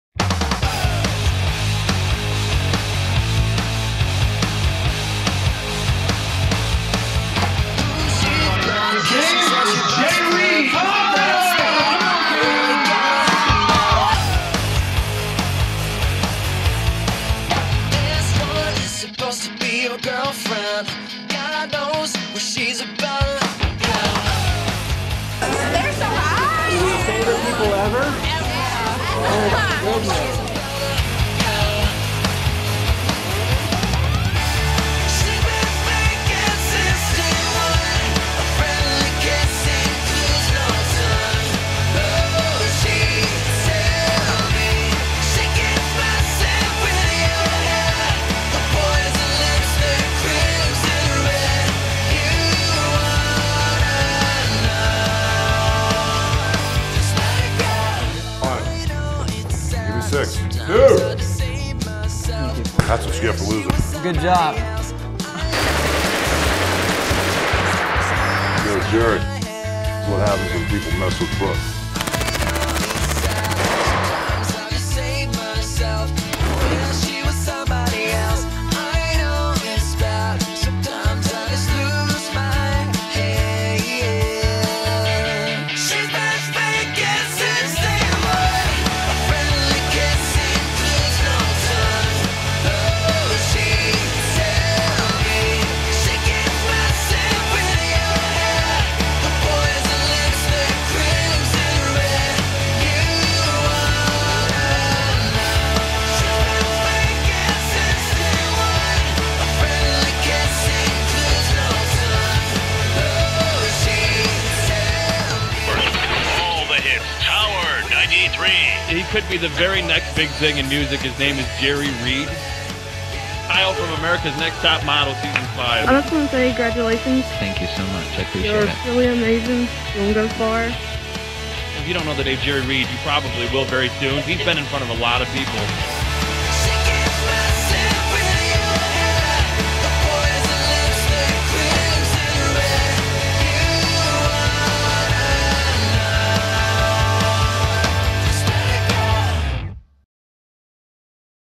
J Reid Promo
Promo video for music artist Jerry Reid.  This is strictly an editing project as all footage and photos were provided to me.